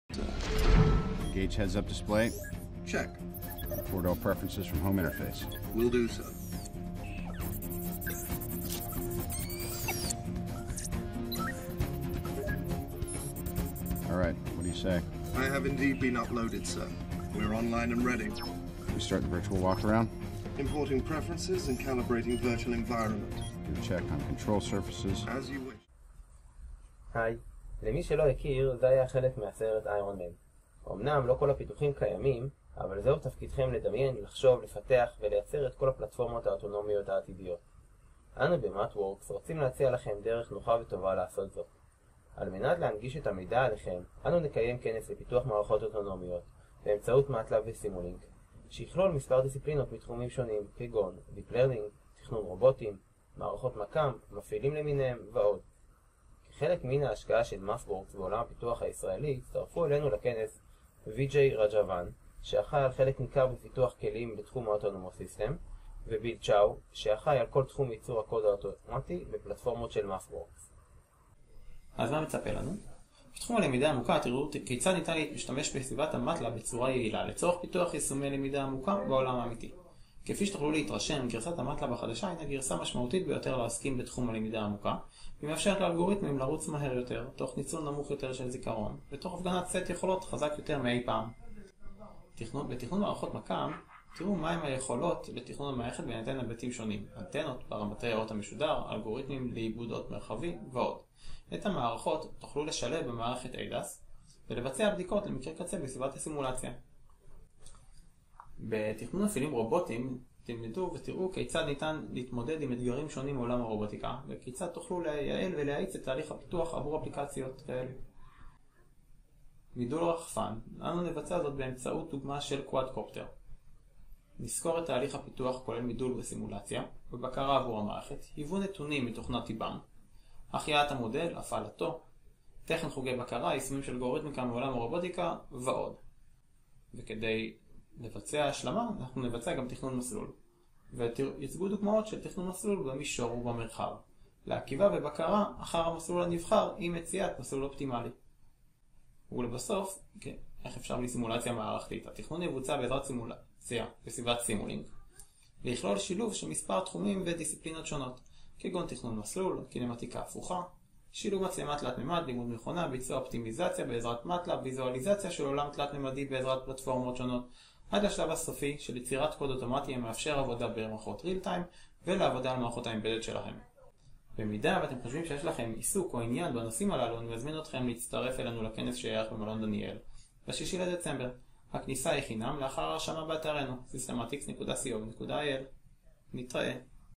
6.12.17 הזמנה לכנס פיתוח מערכות אוטונומיות באמצעות MATLAB ו- Simulink
כנס טכני במלון דניאל הרצליה, ללא עלות, למהנדסים ומפתחים – בו תוכלו ללמוד על תכנון, בדיקה ויישום של מערכות אוטונומיות ומערכות תומכות למערכות אוטונומיות.